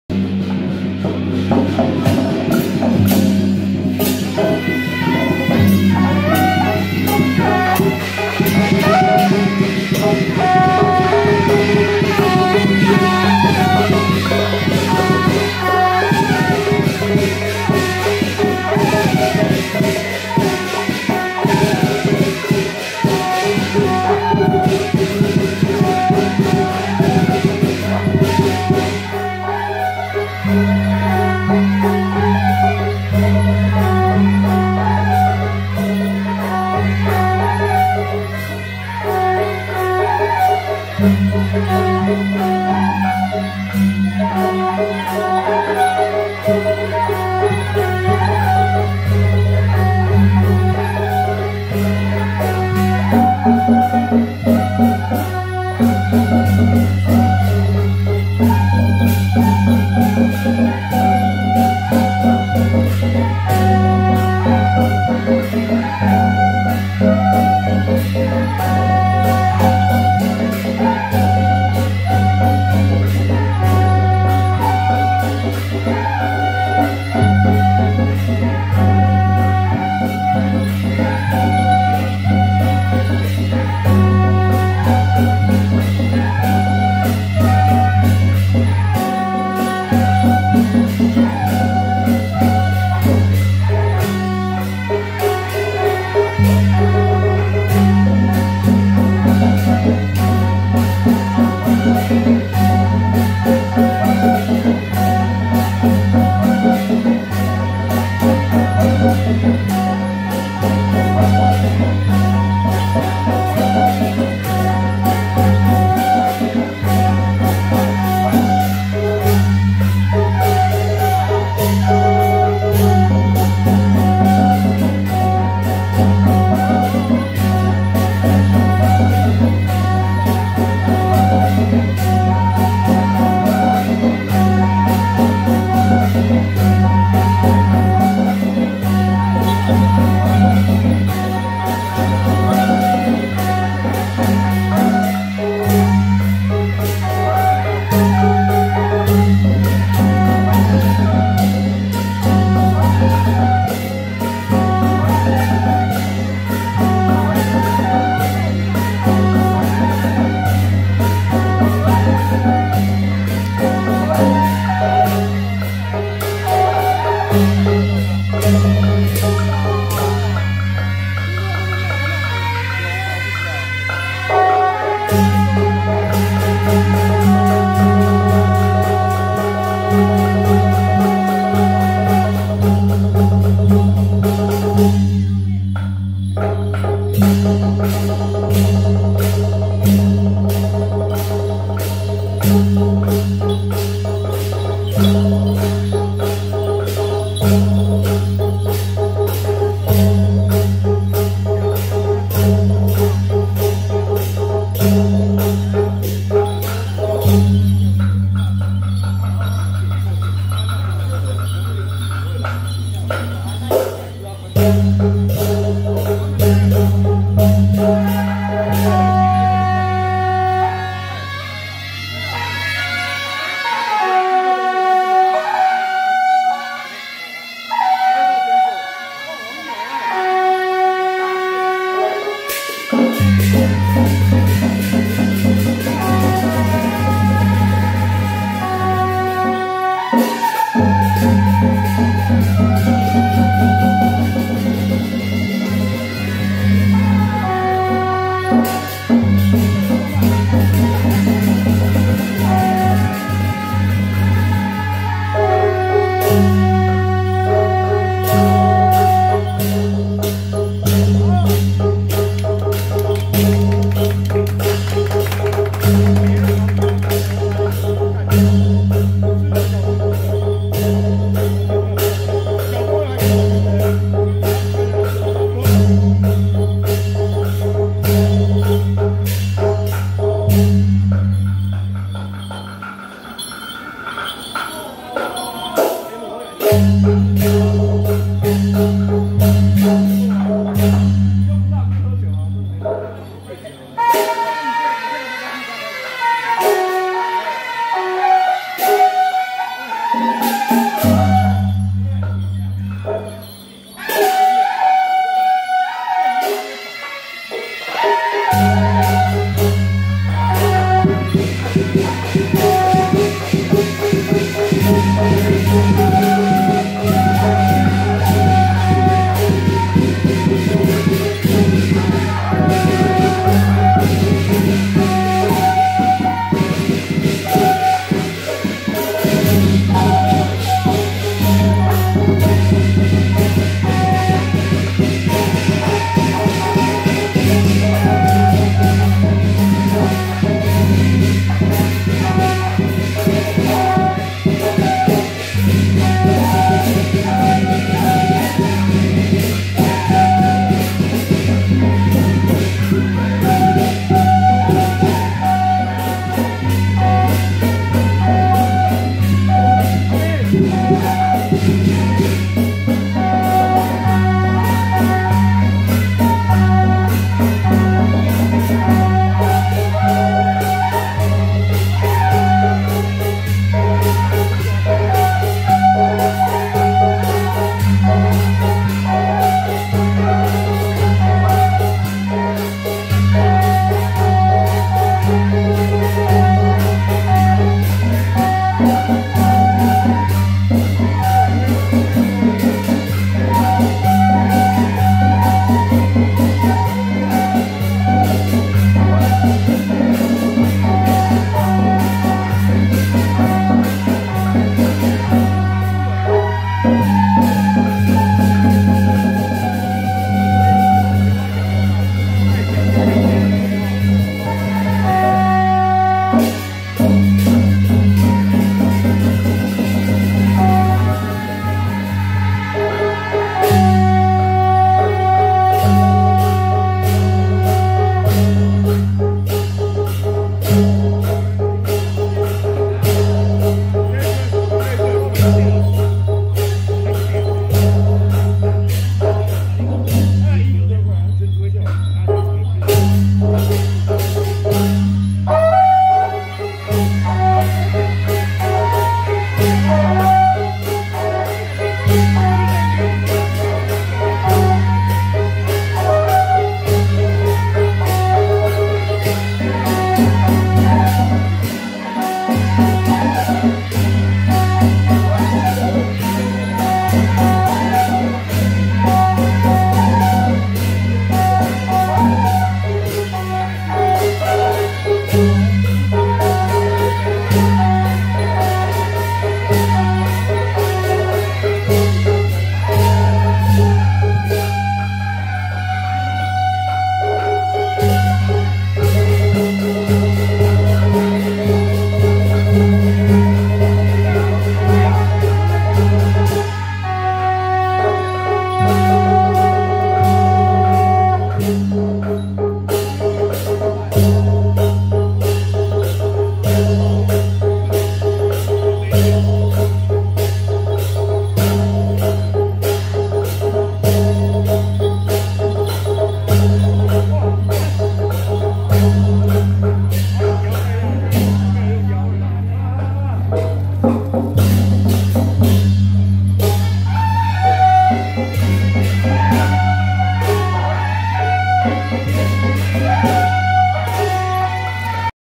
【臺北城城北龍雲堂+南港昭南宮-神將團】~111臺北城城北龍雲堂趙聖帝君南巡謁祖進香回駕遶境
八德和樂軒-北管＆臺北城城北龍雲堂+南港昭南宮-神將團